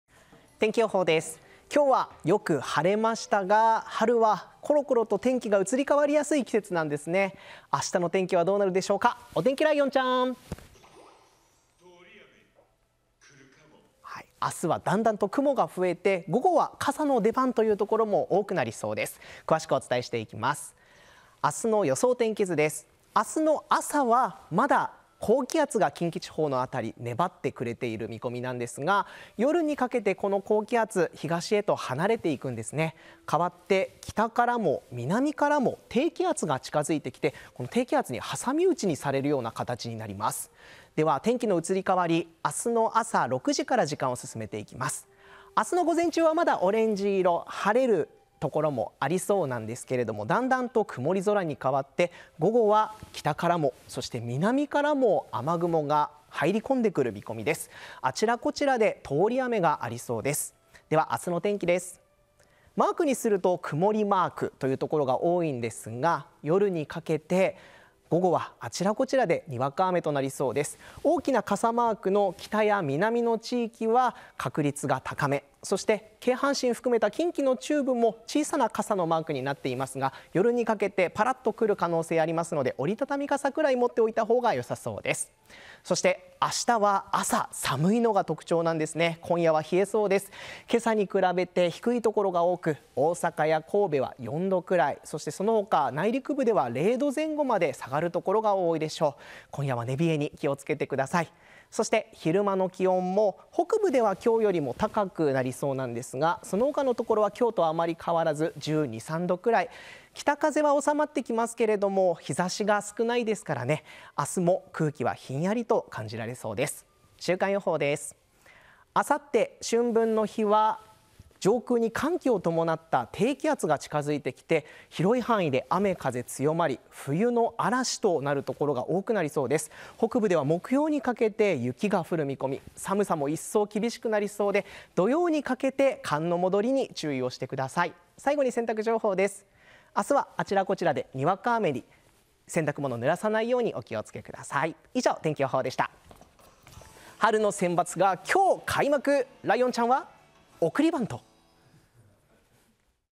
【3月19日(火)】午後にあちらこちらで通り雨 今週は長めの「寒の戻り」に【近畿地方の天気】天気
３月１９日（火）の近畿地方は、午後にあちらこちらで通り雨があるでしょう。雨具を持ってお出かけください。

　天気の移り変わりが早く、晴れをもたらす高気圧は次第に東へ離れ、低気圧が北からも南からも近づいてくるでしょう。このため、近畿地方は午前中に晴れ間もありますが、昼すぎ以降は所により雨が降る見込みです。特に北部や南部が降りやすくなりそうですが、京阪神を含む中部も雨の可能性がありますので、空模様にお気を付けください。

　朝の冷え込みが強く、最低気温は大阪と神戸で４℃、そのほか内陸部は多くの所で０℃前後まで下がる見込みです。日中の最高気温は北部では前日より高くなりますが、中部や南部は概ね前日と同じくらいで、１２～１３℃という所が多い見込みです。日ざしも少ないため、昼間もひんやり感じられるでしょう。

　２０日（水）の「春分の日」は上空に強い寒気を伴った低気圧が近づき、大気の状態が非常に不安定になる見込みです。雨や風が強まり、雷や突風を伴うおそれもあります。２１日（木）にかけて寒さも一層厳しくなり、北部では広く雪が降るでしょう。冬の嵐の再来となり、春の彼岸のお墓参りやお出かけにはあいにくの天気になってしまいそうです。彼岸明けの２３日（土）にかけて平年より気温が低めで、今週は長めの「寒の戻り」となる見込みです。